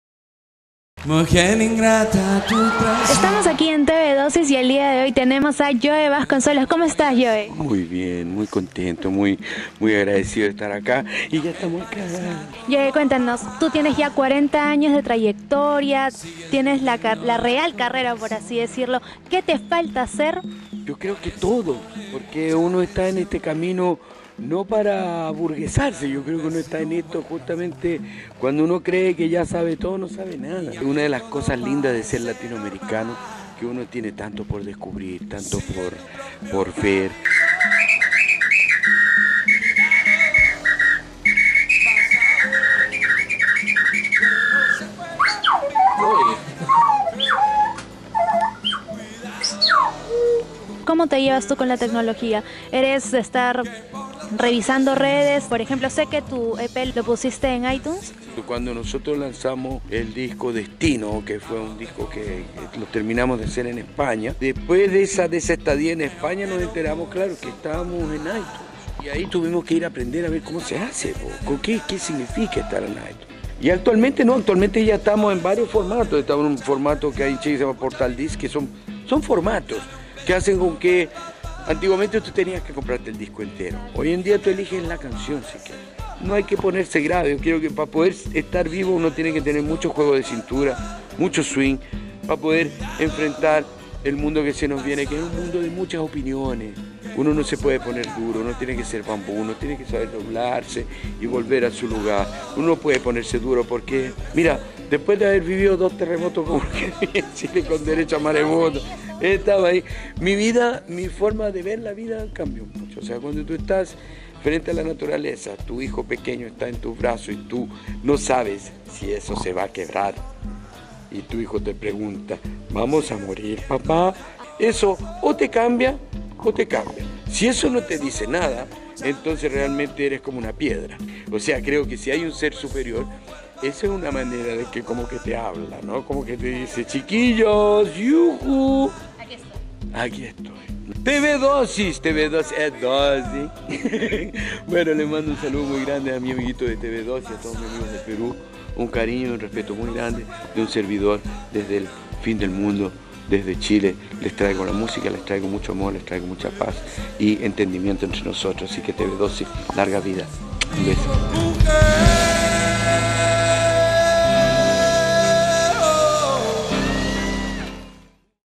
TVDOSIS: JOE VASCONCELLOS
Entrevistamos a Joe Vasconcellos en su paso por Lima y conversamos con él acerca de la música, la tecnología y la vida.